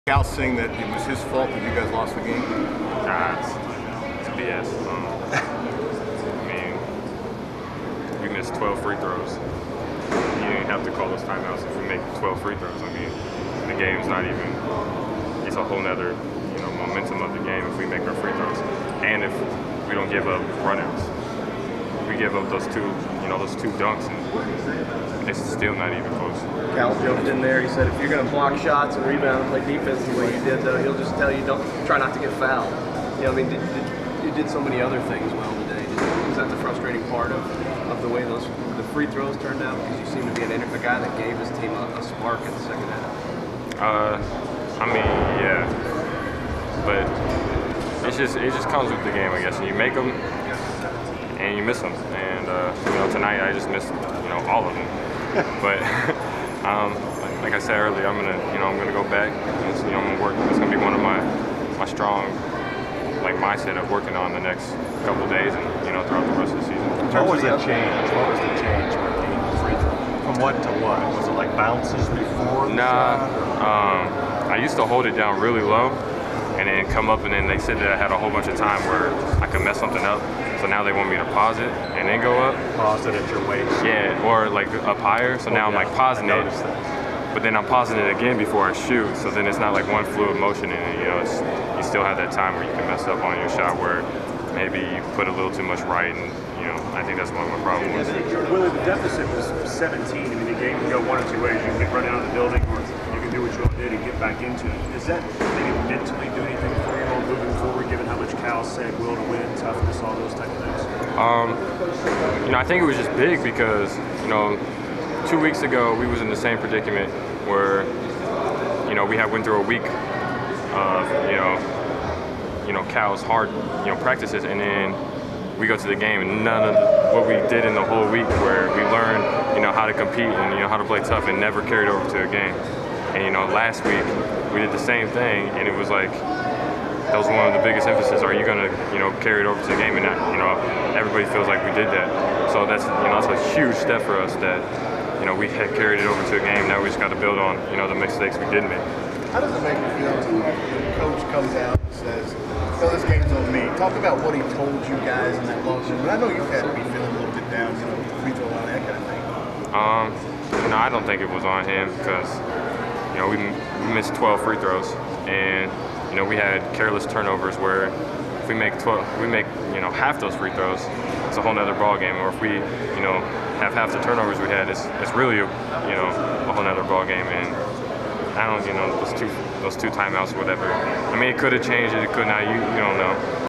Kentucky Wildcats TV: Willie Cauley-Stein Louisville Postgame
Willie Cauley-Stein had 6 points and 8 rebounds.